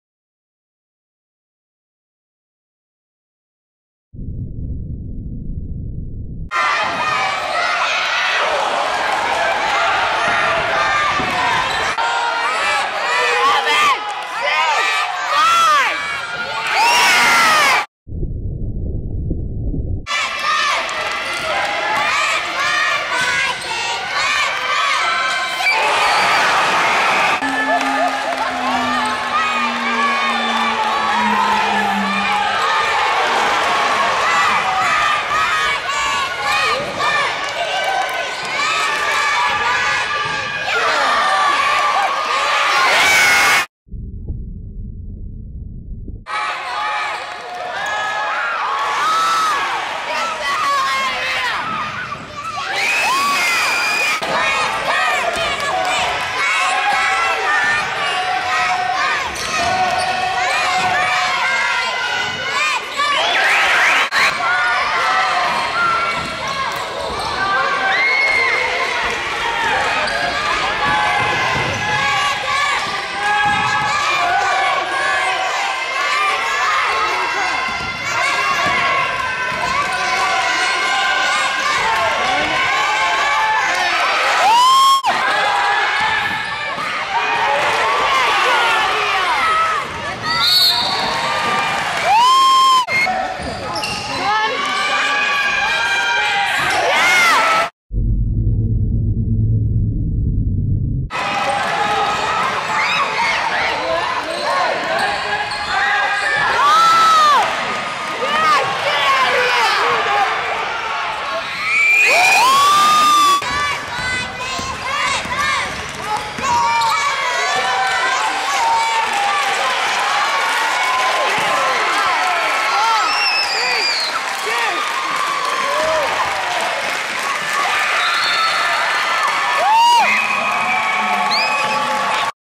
James Janssen heroic last quarter.
James Janssen heroically closes out the semi-final against the Keysborough Cougars.